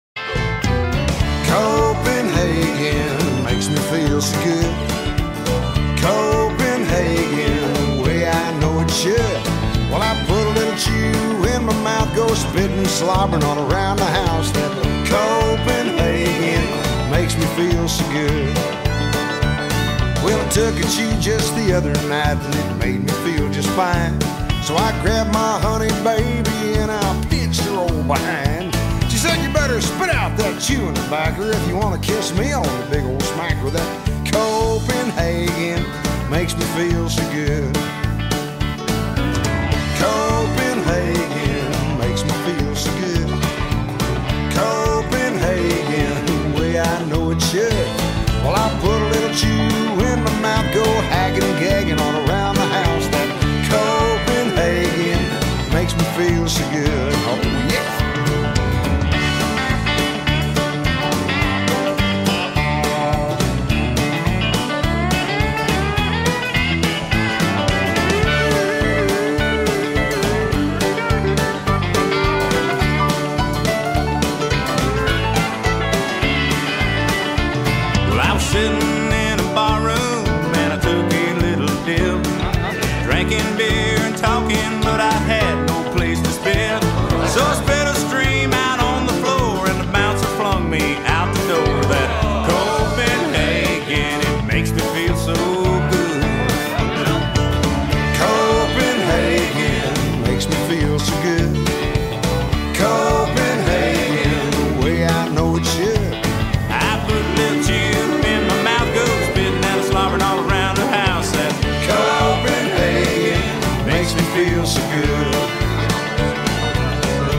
Beautiful City
The beautiful capital of Denmark on a nice summer day!